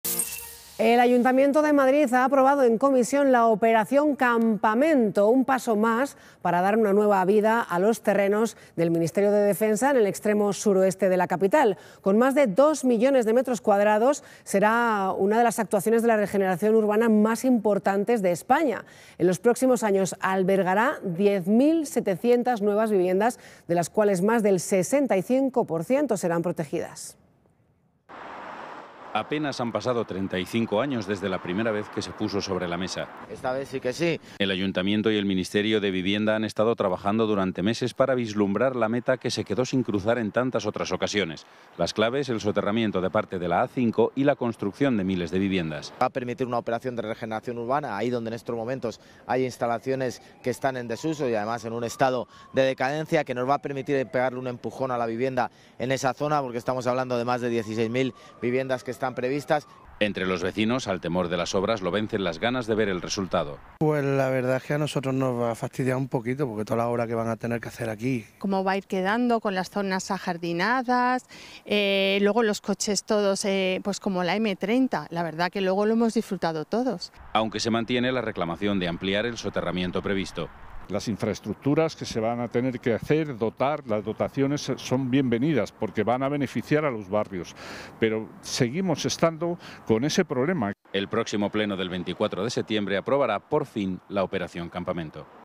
Madrid da el penúltimo paso de la Operación Campamento
Un total de 10.700 viviendas sobre una actuación urbanística de más de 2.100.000 metros cuadrados... Estas son las cifras de la Operación Campamento.

El Ayuntamiento de Madrid ha dado el penúltimo paso para desbloquear la operación con la tramitación del proyecto de urbanización aprobando la iniciativa de gestión urbanística del Ministerio de Vivienda y Agenda Urbana, formalizada por Sepes.